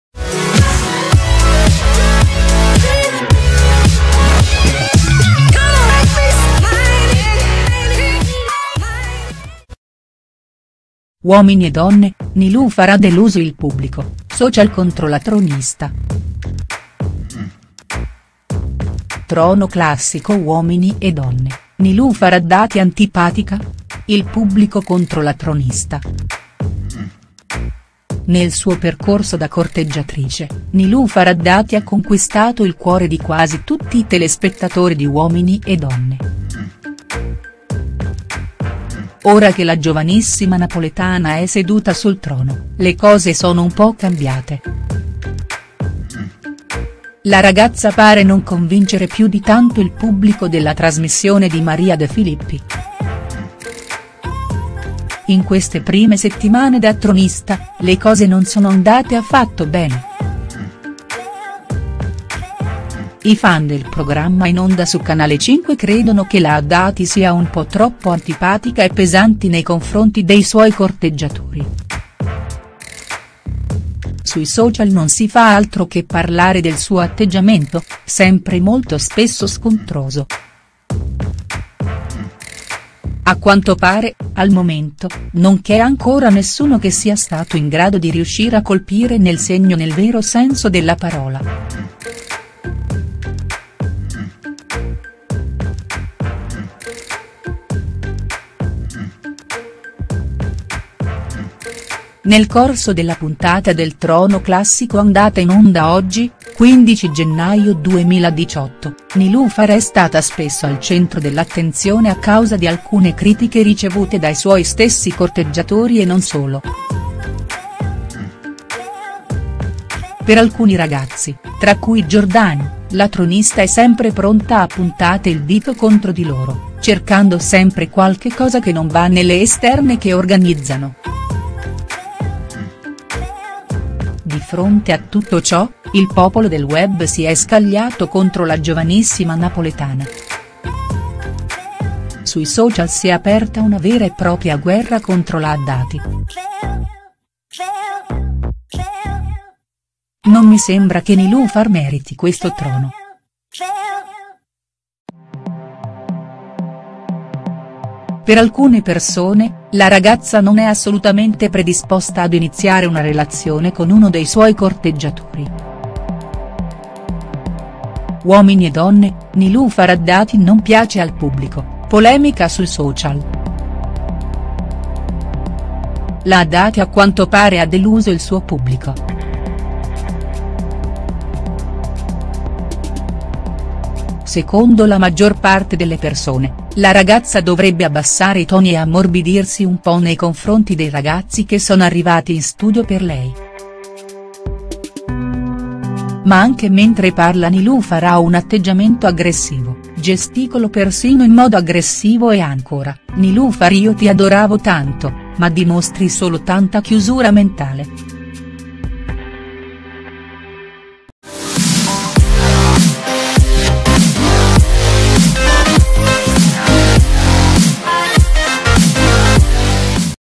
Uomini e Donne, Nilufar ha deluso il pubblico: social contro la tronista| STARS NEWS
Il pubblico di Uomini e Donne contro Nilufar Addati. I fan non approvano l'atteggiamento della giovanissima tronista del Trono Classico!